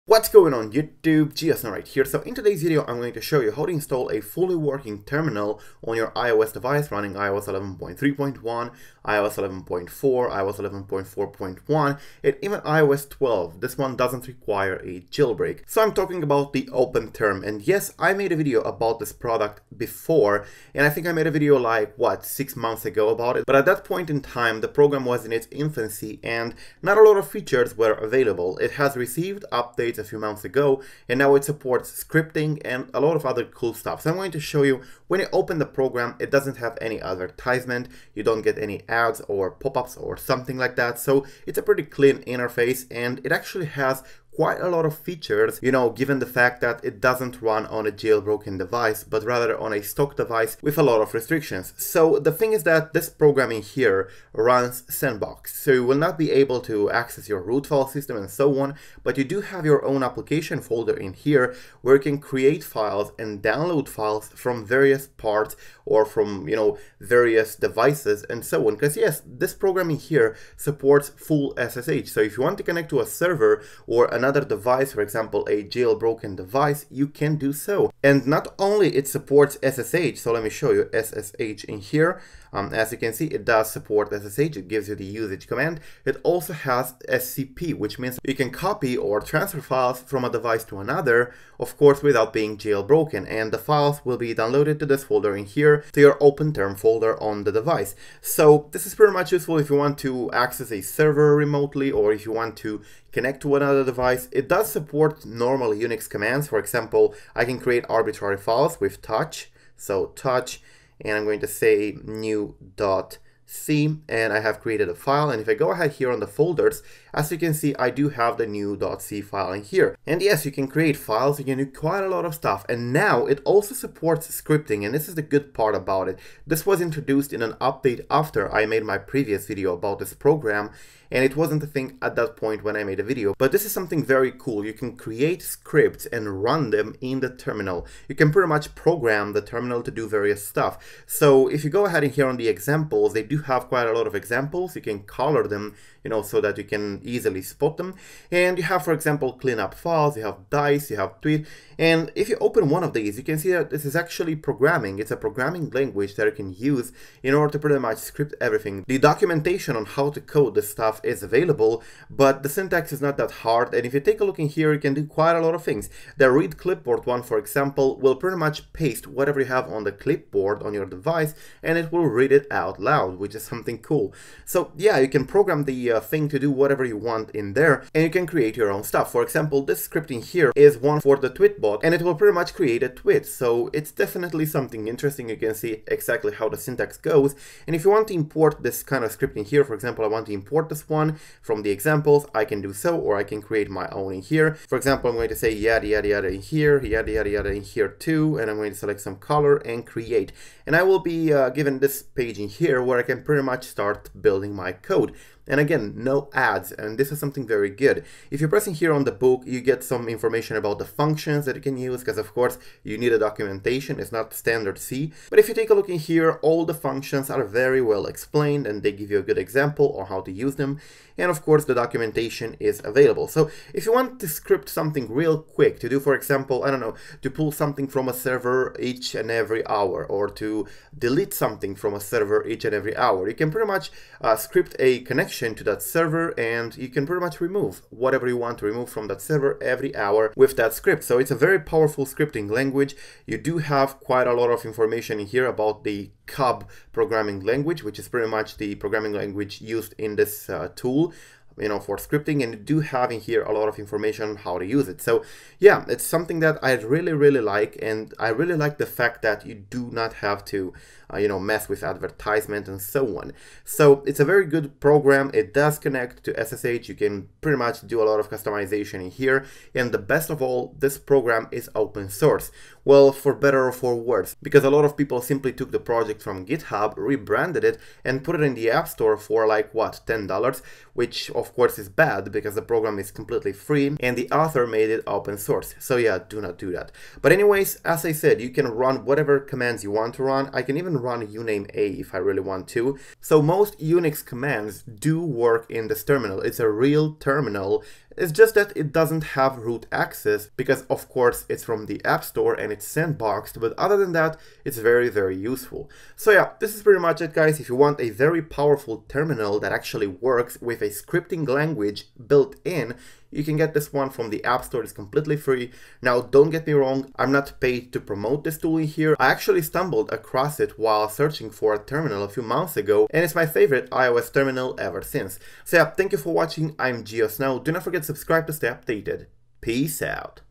iOS 12 / iOS 11.4.1 / 11.4 Full TERMINAL With SSH and Scripting (NO Jailbreak)
In today's video, I am going to show you how to get a fully functional iOS Terminal without a jailbreak on iOS 12 Beta, iOS 11.4.1, iOS 11.4 and even lower. This Terminal called OpenTerm is an open source application available in the App Store for free compatible with iOS 11.x devices. The terminal supports most Unix commands you'd find on a jailbroken device or on a Mac, and it does include SSH and SCP support which means that you can use it to connect remotely to a jailbroken device or to a server and even transfer files back and forth from your device.

The terminal also has a scripting language built-in which allows you to make pretty powerful scripts that you can run in the terminal. OpenTerm also includes the necessary documentation for the Cub Programming language used for scripting and it is a quite easy syntax, somehow similar to C or C++. The Terminal runs sandboxed since it is an App Store application so you will not be able to access the ROOT file system with this. It's very useful if you want an SSH or SCP interface to communicate with your server from your device tho.
It can also create and edit arbitrary files using the built-in Unix commands.

OpenTerm is open source which means that anybody can contribute to the project and add more features, so if something is missing and you'd like it added, you can definitely check out their GitHub project page. However, some bad-intentioned individuals have changed the branding of the app and re-uploaded it to the App Store and are charging for it. Avoid those as the OpenTerm is free.